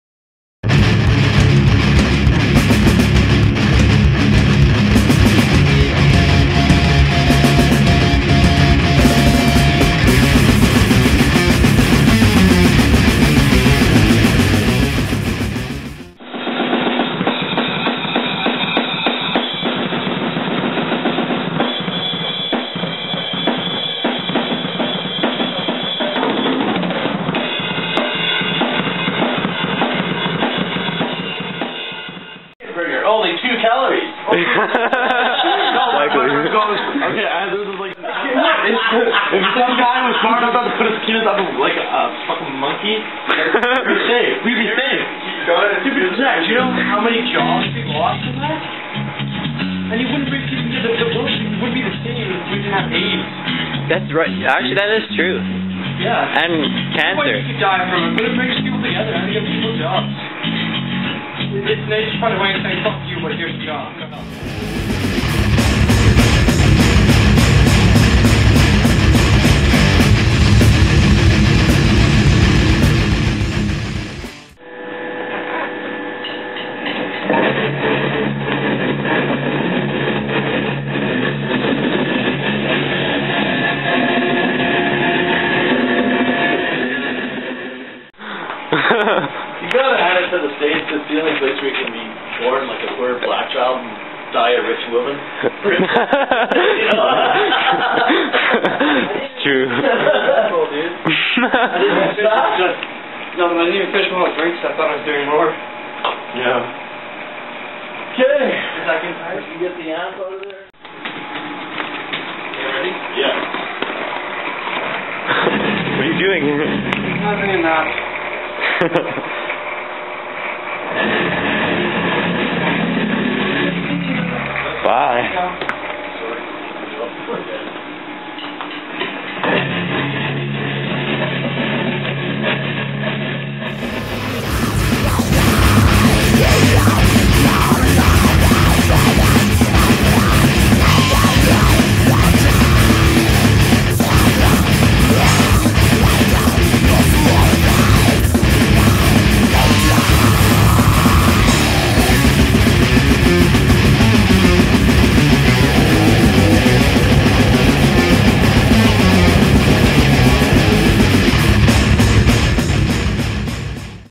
Under The Dead Sky In The Studio
Under The Dead Sky in the studio recording their first demo.

Zach Hopkins: Vocals
Sheldon Ouellette: Drums
Aaron James: Lead Guitar
Alex Labbé: Rhythm Guitar

Buy the new Under The Dead Sky CD now for the low price of 5 dollars!